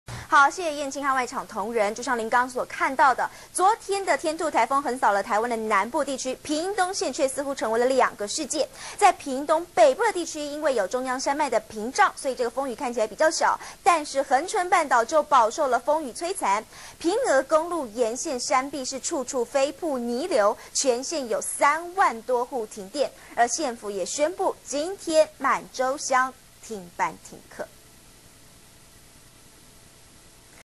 中視新聞》海水倒灌滿州鄉 珊瑚海藻衝上岸
中視新聞》天兔颱風暴風圈21日掃過台灣南部，屏東縣變成2個世界！屏北地區因有中央山脈屏障，風雨較小；恆春半島則飽受風雨摧殘，屏鵝公路沿線山壁處處飛瀑、泥流，全縣有3萬多戶停電。縣府宣布，22日滿州鄉停班停課。